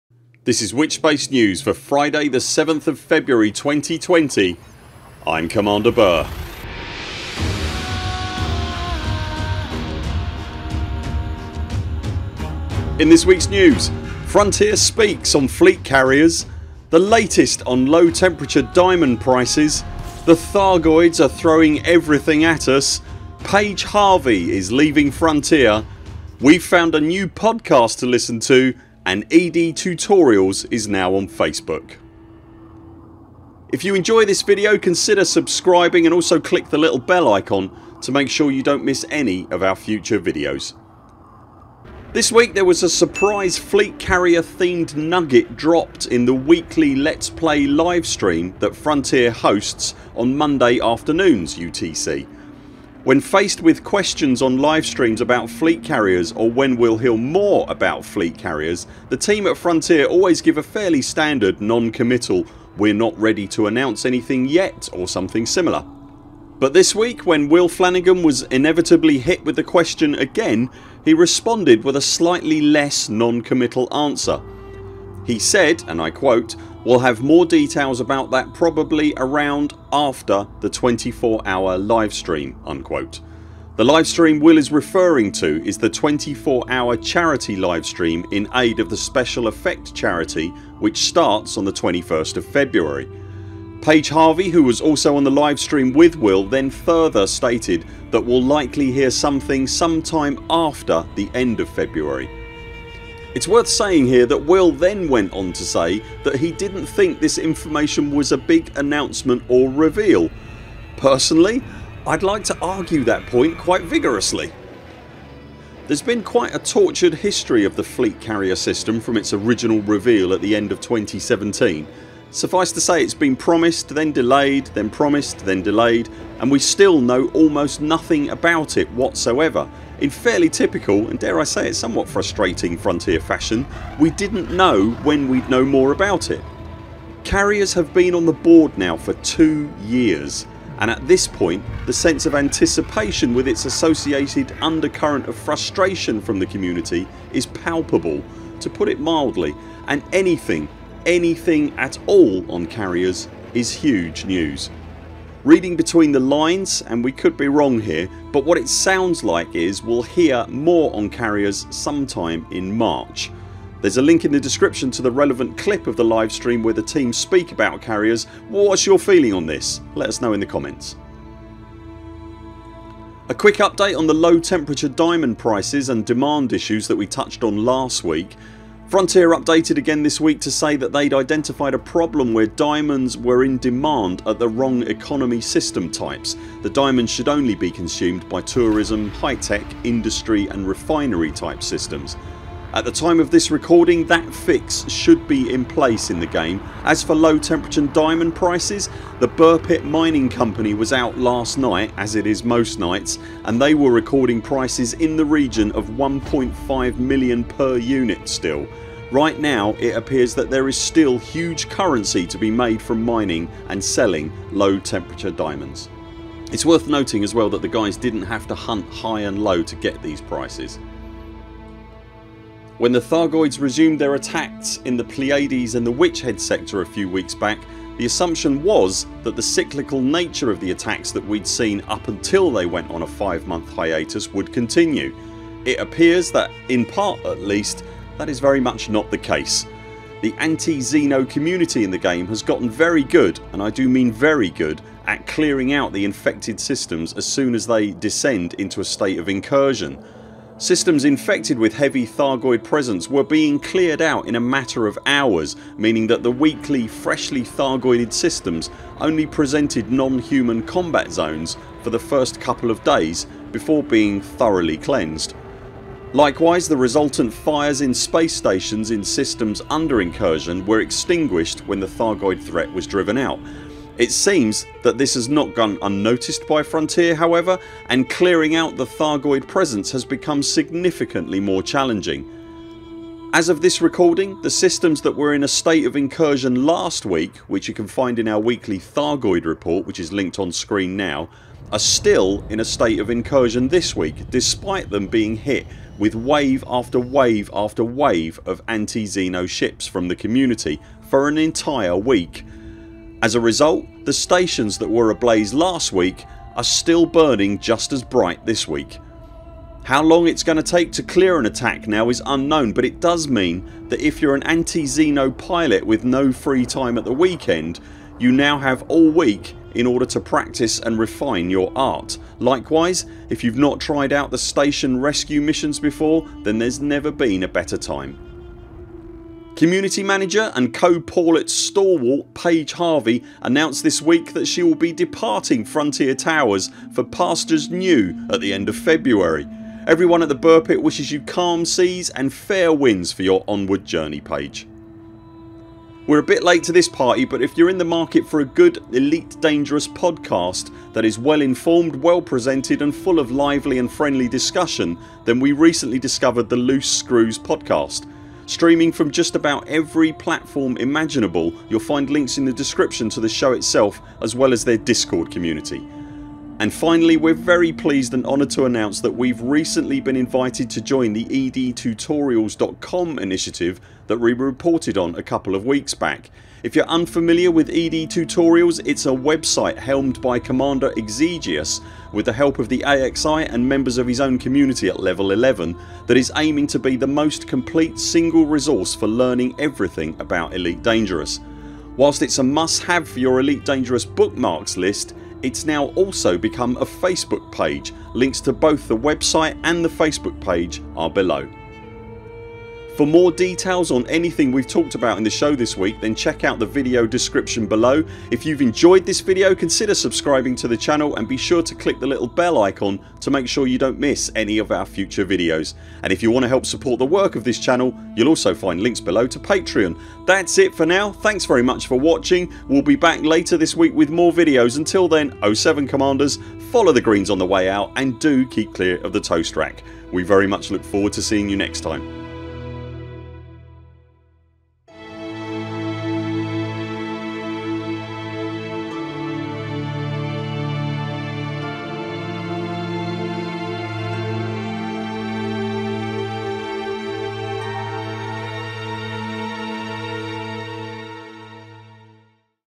Elite Dangerous: Witchspace News FDEV on Carriers, Low Temp Diamonds & more | 7th of February 2020
Elite Dangerous: Witchspace News for the 7th of February 2020

Frontier reveals when we’ll likely hear more on Fleet Carriers, the Thargoids push back, we update on the Low Temperature Diamond market and more…